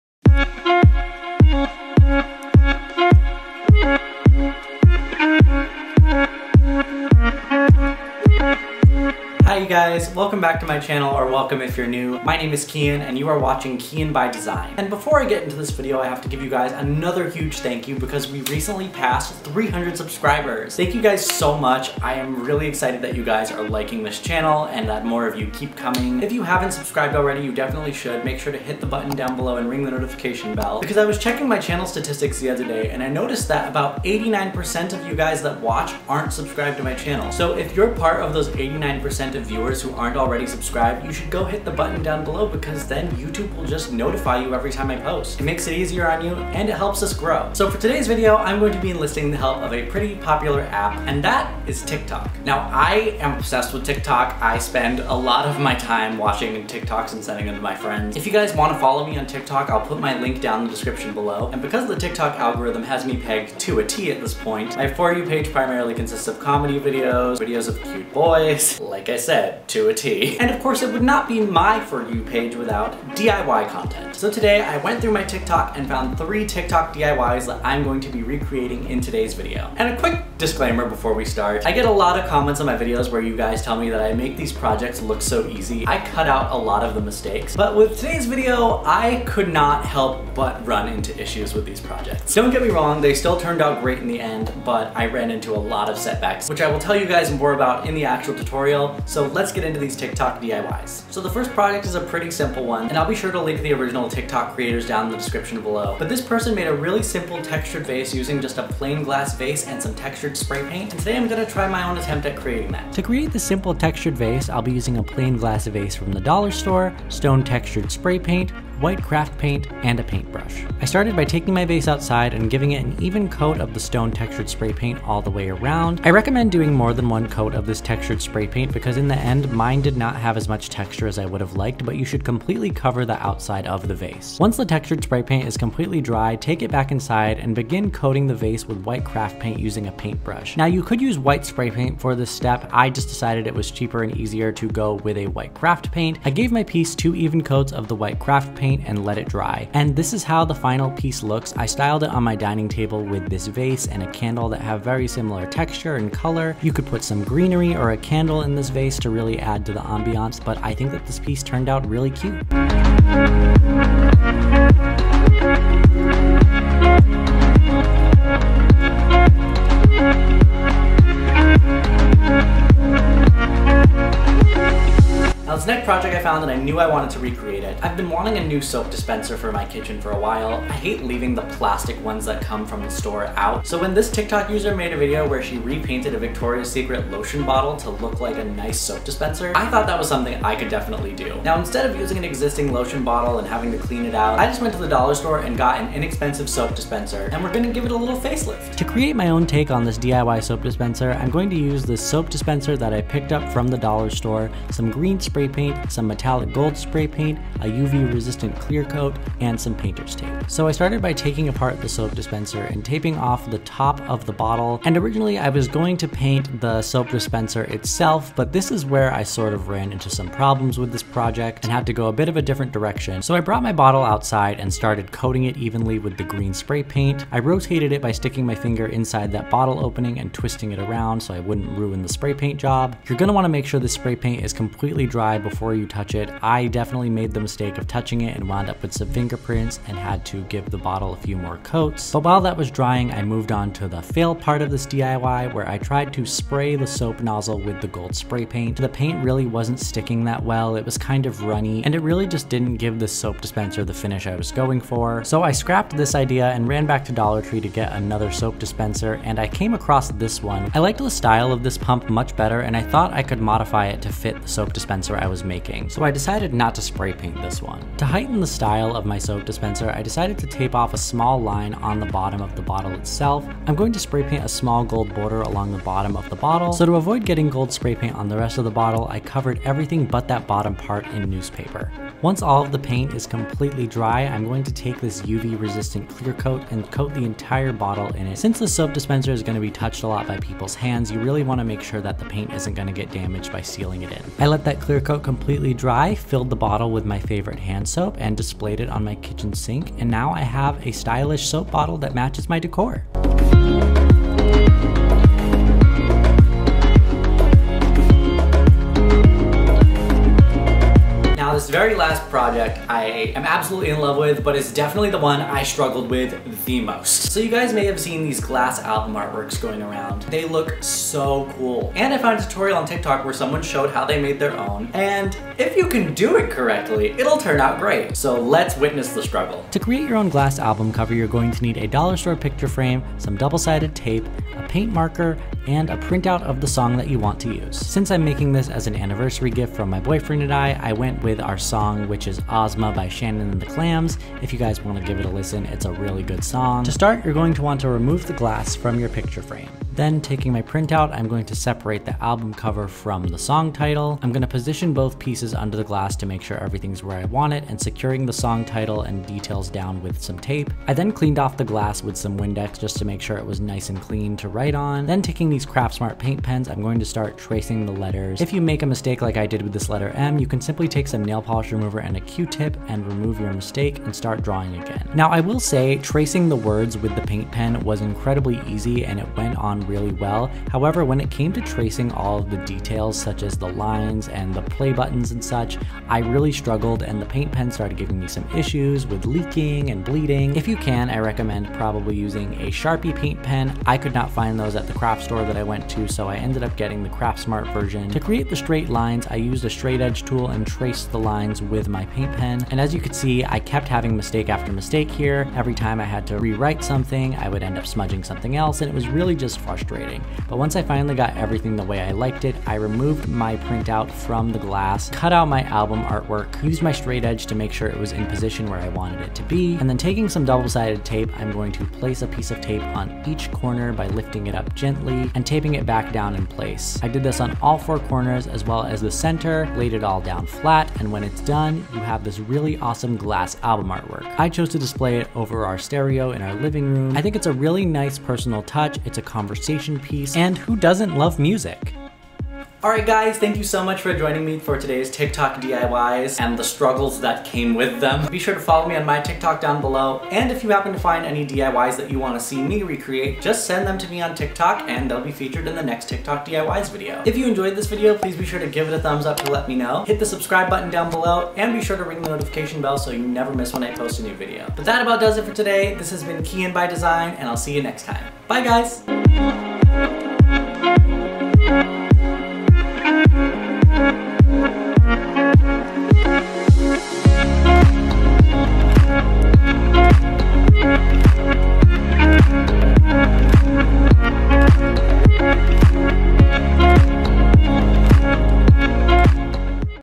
Trying TikTok DIYs! - Kyian By Design
Look who's (Tik)Toking now! If you love Tiktok as much as I do, you'll love these TikTok DIYs I tried in today's video!